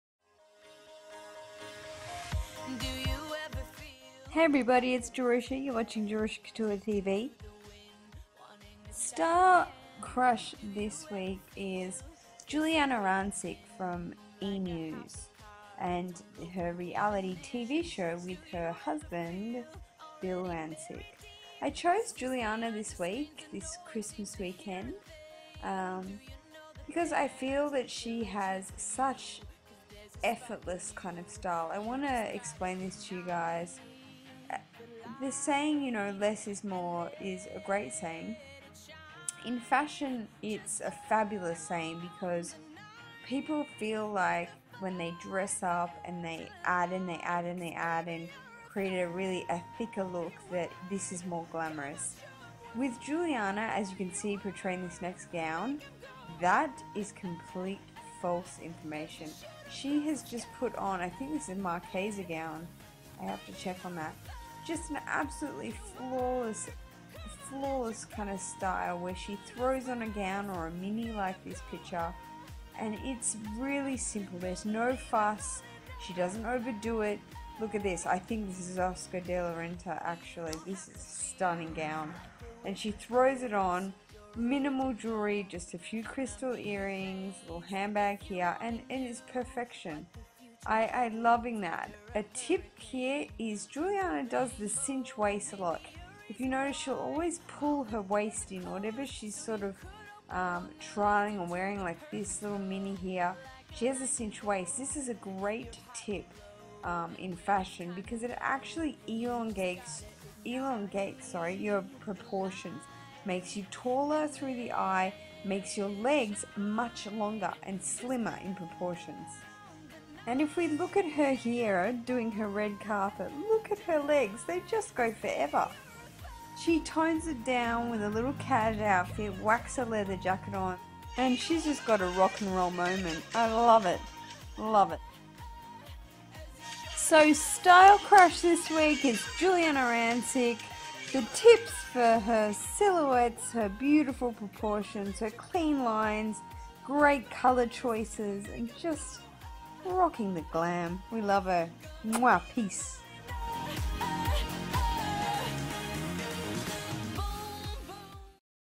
Weekly Style Crush-Giuliana Rancic
Weekly Style Crush for this Christmas weekend is Giuliana Rancic.
I admire her effortless glamour and clean lines.
X
Jerusha

Giuliana Rancic
-Glamorous
-Clean lines
-High cinched waist
-well combine colour selections